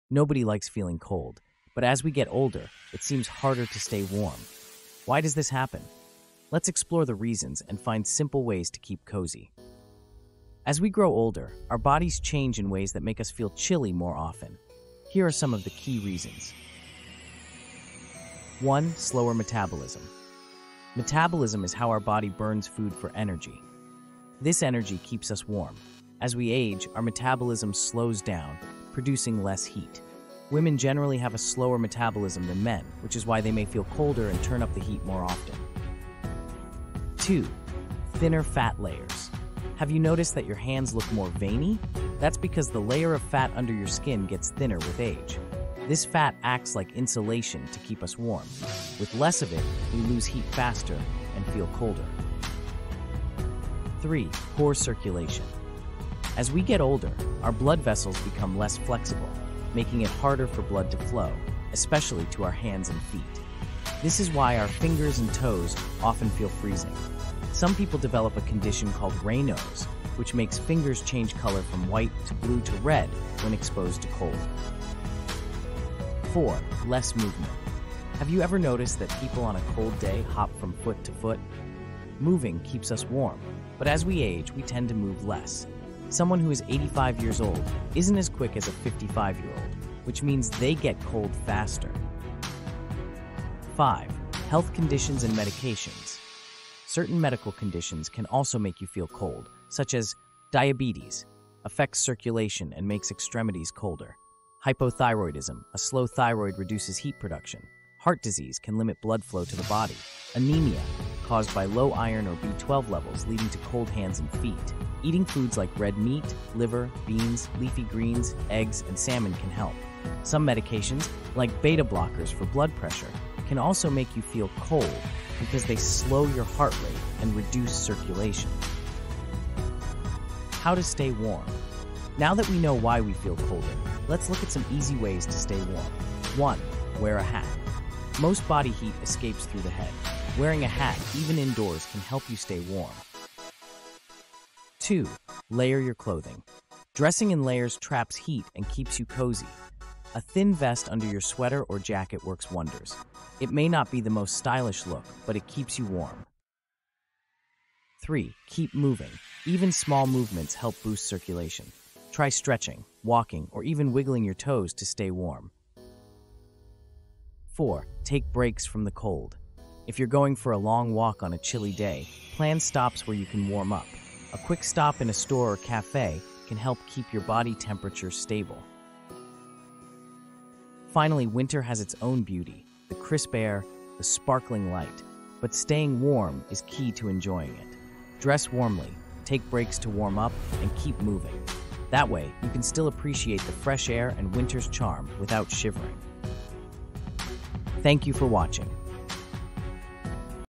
Why Do We Feel Colder as We Age?
Why do we feel colder as we age? In this engaging video, we explore the intriguing factors that contribute to our sensitivity to temperature as we grow older. Learn about the physiological changes that affect our body’s ability to retain heat and what you can do to stay cozy.

Don’t miss out on these valuable insights!